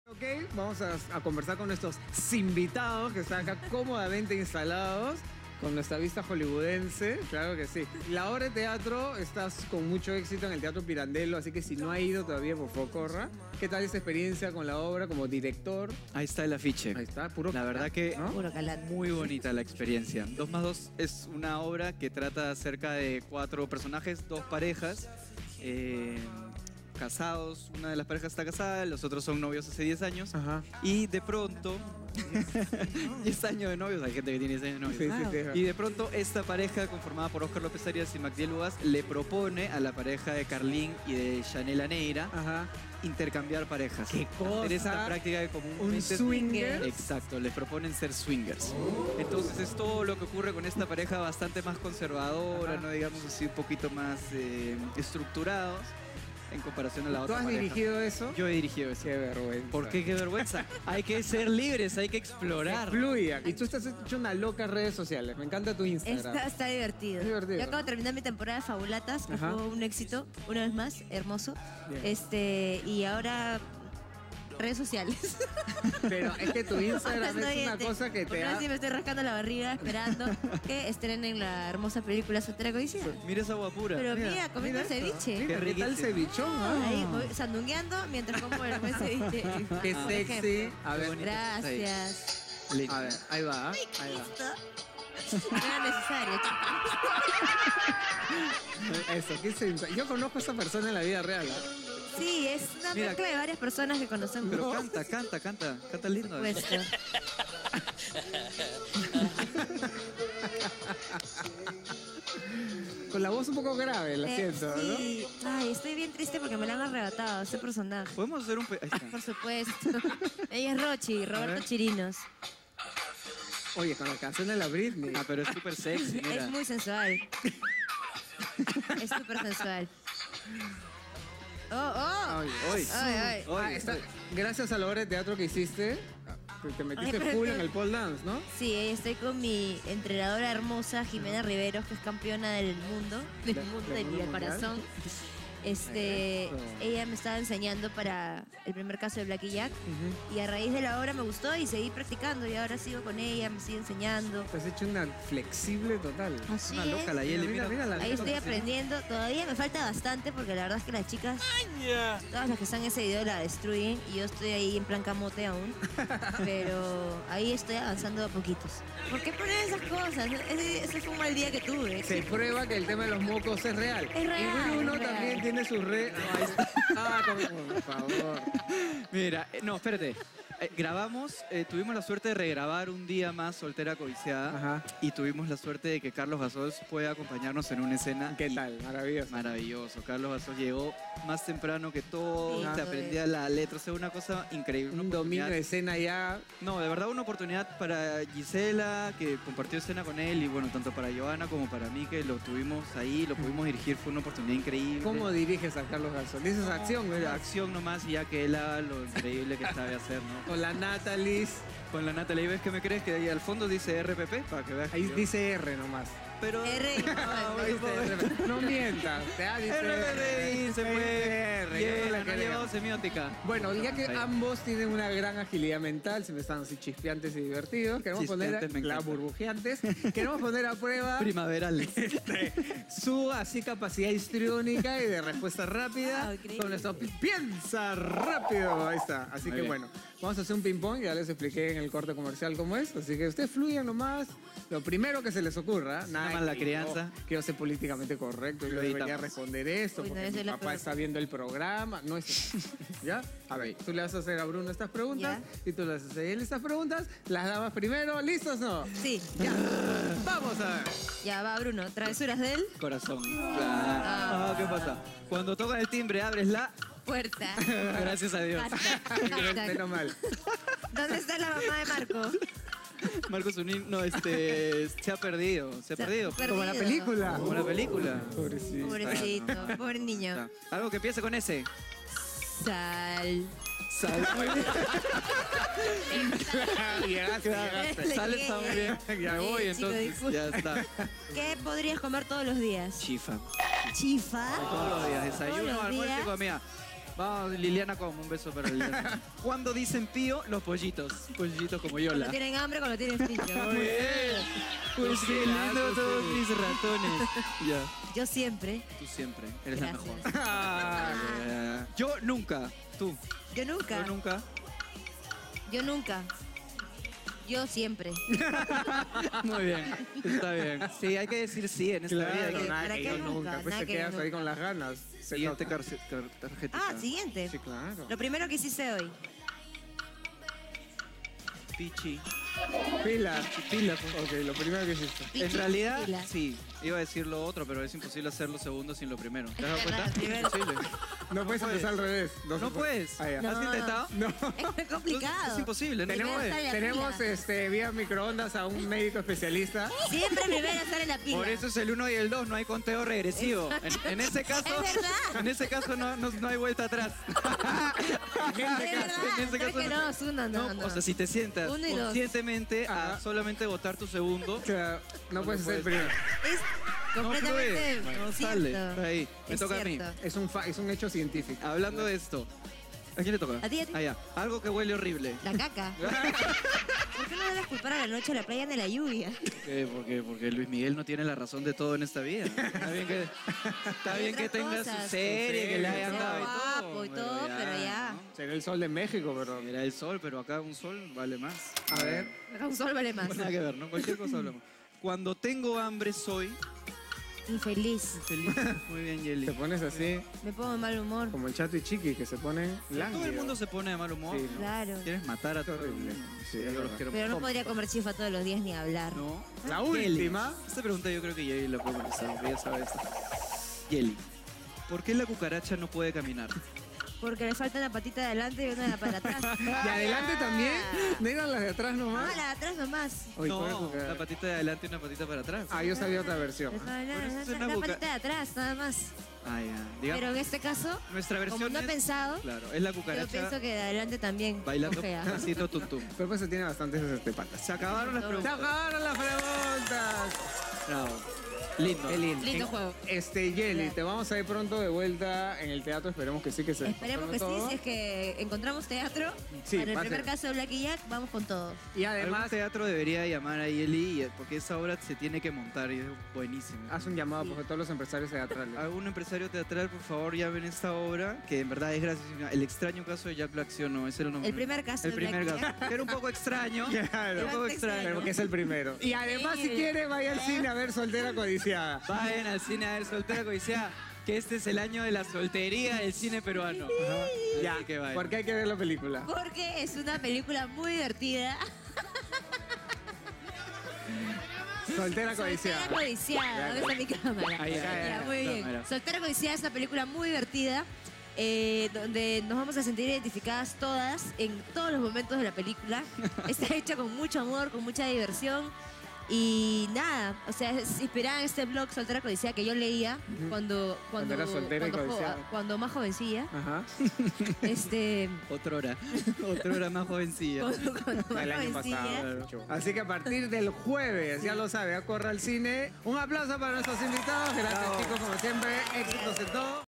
'Soltera Codiciada': Entrevista + Juego / Jely Reategui y Bruno Ascenzo
¿Recuerdan que les contamos que Jely y Bruno vinieron a acompañarnos al set? Pues decidimos hacer un pequeño juego con ellos. ¡No te lo pierdas!